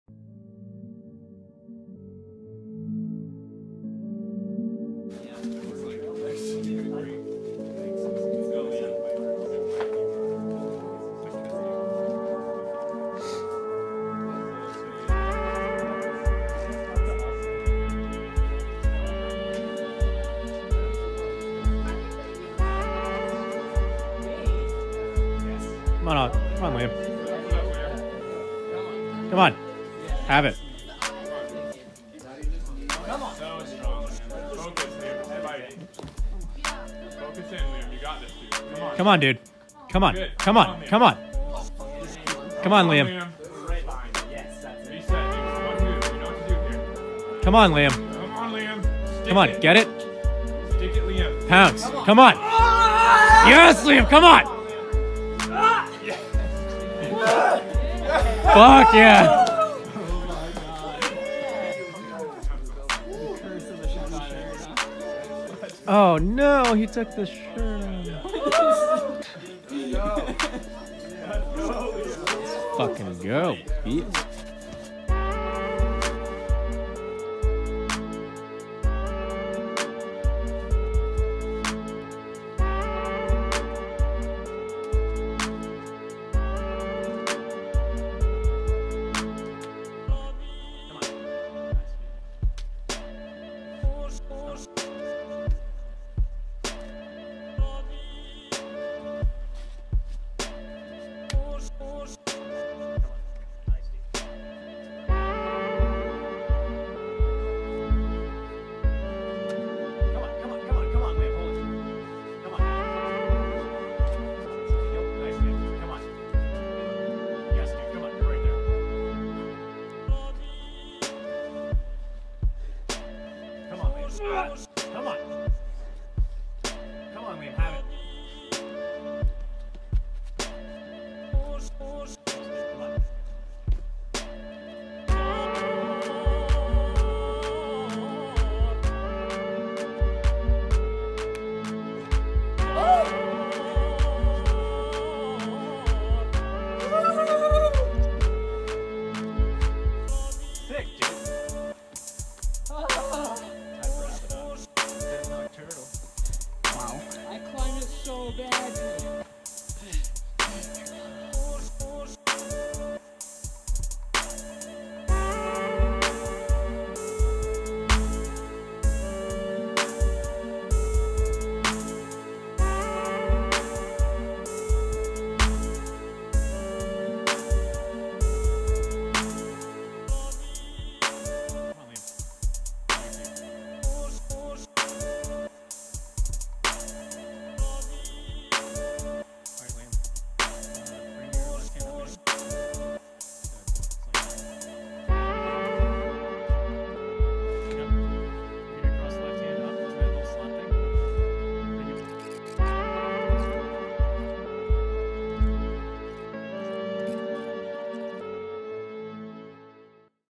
Do Boulders Get Better Than These? CWU Extra: Liam Does The Black Velvet Trifecta!
Today we're throwing it back to a weekend in January, when ClimbWithUs vlogger Liam Andrews-Bancroft and CWU affiliate Tristan Chen travelled to Red Rocks, Nevada to attempt 3 of the most amazing problems in the world: Wet Dream (v12), Atlas Shrugged(v12), and Abaddon(v11/12). The boys were lucky enough to get them done, and today we're sharing the uncut  footage of Liam climbing all 3!